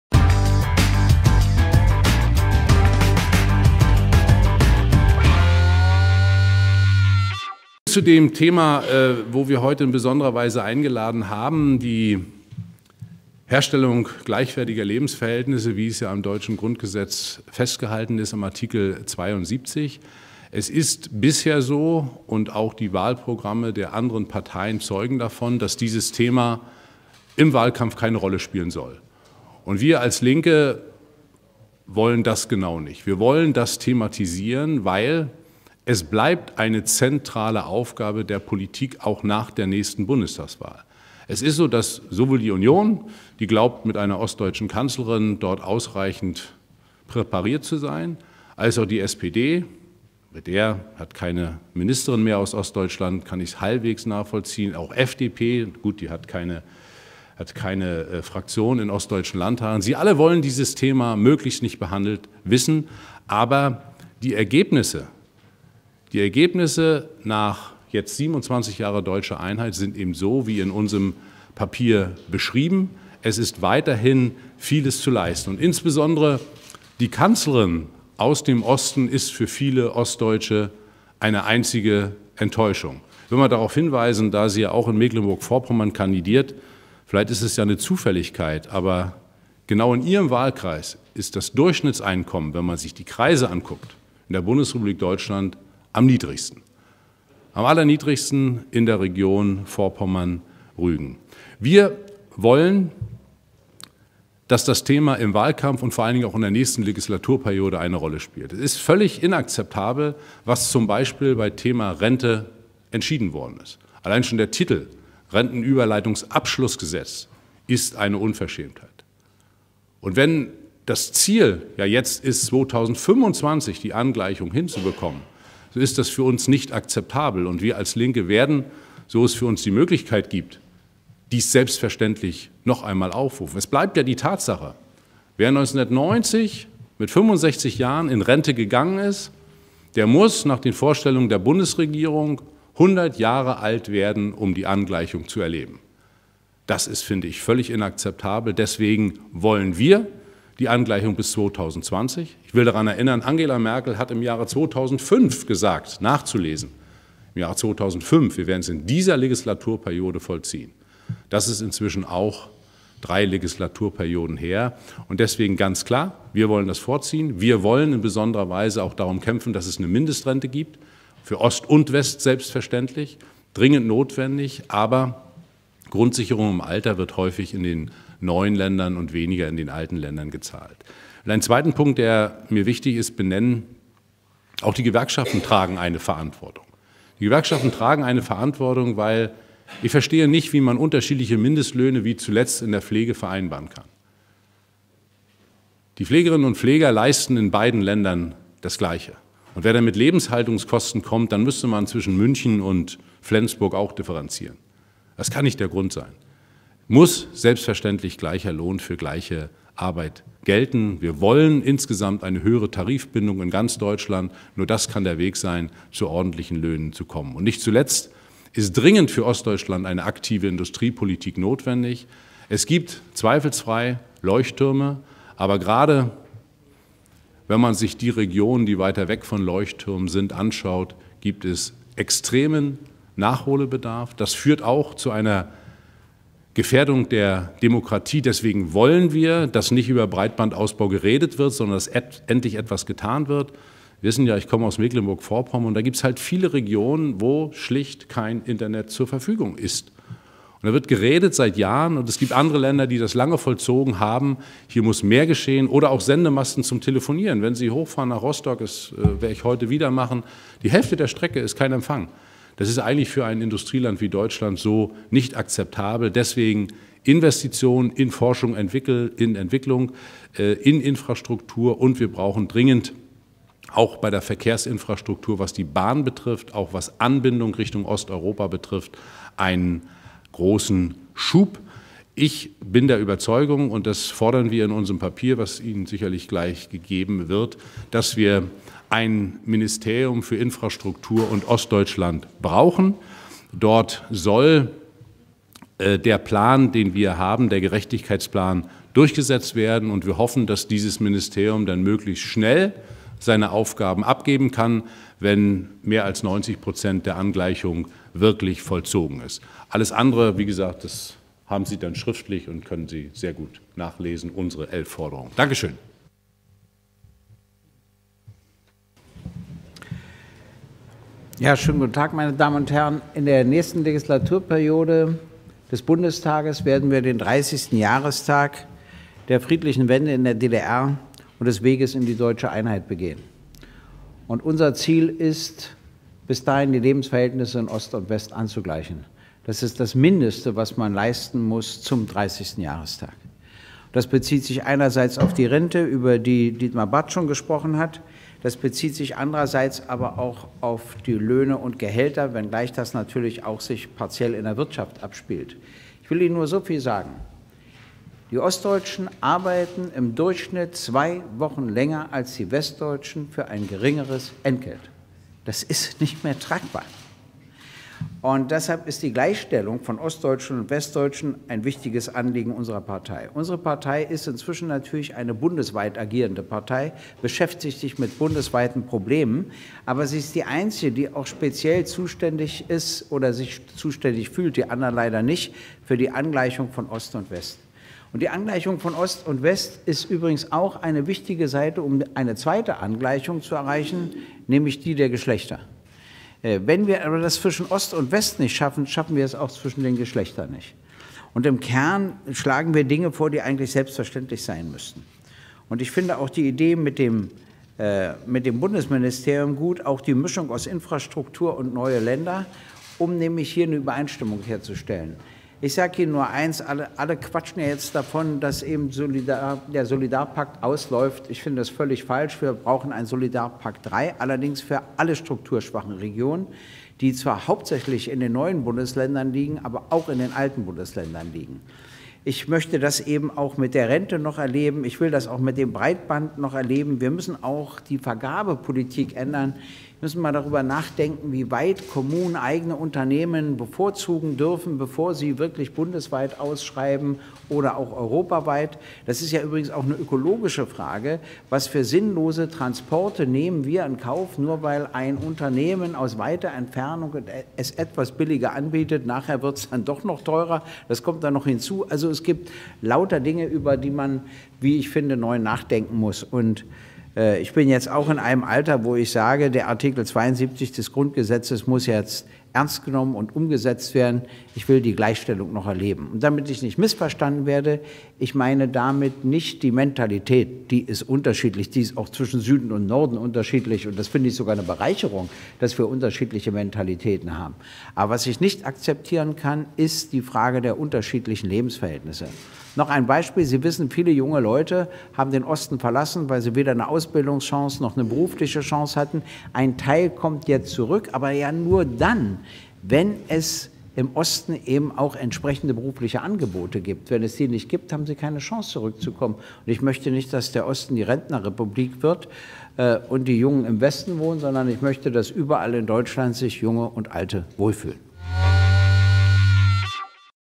„Allianz für Ostdeutschland“ für gleichwertige Lebensverhältnisse
Dietmar Bartsch, Spitzenkandidat der LINKEN zur Bundestagswahl und Gregor Gysi, Präsident der Europäischen LINKEN und Direktkandidat im Wahlkreis Treptow-Köpenick, übten bei der Vorstellung der Initiative der LINKEN für die Schaffung gleichwertiger Lebensverhältnisse in Deutschland „Allianz für Ostdeutschland“ im Rahmen der Pressekonferenz scharfe Kritik an der Politik der Bundesregierung. Gerade weil wir in der kommenden Legislaturperiode den 30. Jahrestag der friedlichen Wende und deutschen Einheit begehen werden, braucht es nochmal einen Ruck, um den Verfassungsauftrag zur Herstellung gleichwertiger Lebensverhältnisse umzusetzen und ihn nicht weiter auf den St. Nimmerleinstag zu verschieben.